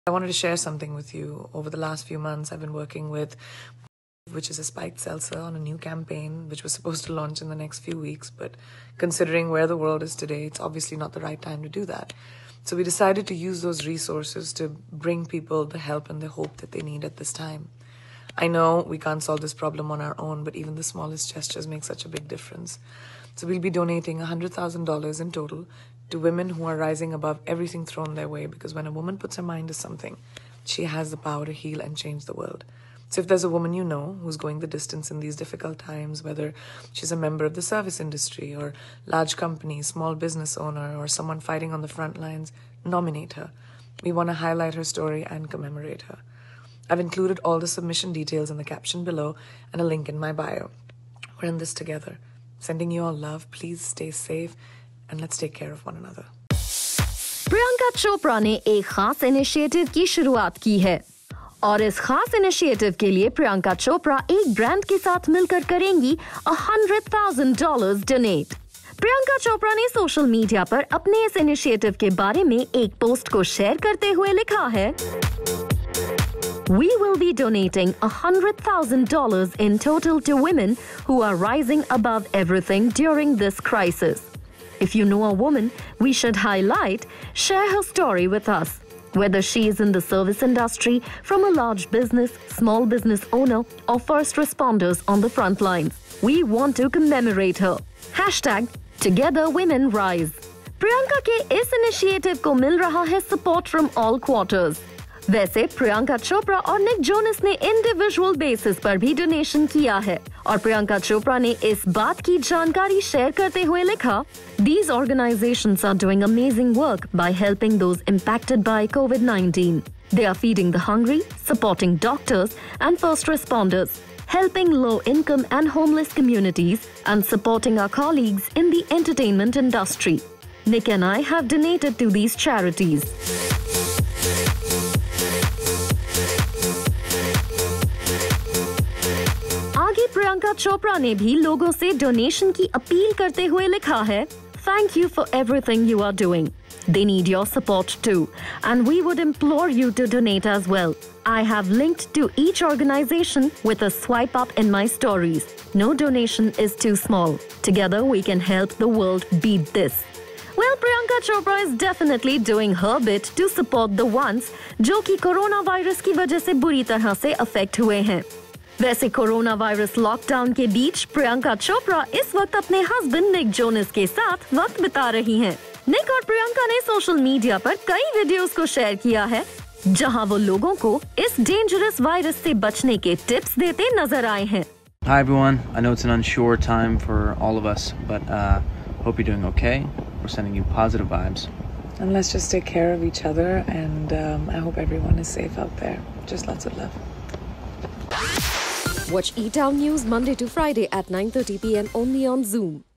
Priyanka Chopra to donate $100,000 with an organisation to women doing their bit amid crisis
Priyanka Chopra Jonas is doing every bit possible to help people amid crisis. Priyanka and Nick have already made donations and now Priyanka makes a special announcement. Priyanka Chopra pledges to donate $100,000 with an organisation to the women who are working towards the betterment of the society amid crisis. Priyanka also issued a statement, which read: "We will be donating $100,000 in total to women who are rising above everything during this crisis. If you know a woman we should highlight, share her story with us, whether she is in the service industry, from a large business, small business owner, or first responders on the front lines. We want to commemorate her. TogetherWomenRise." Watch the video to know more.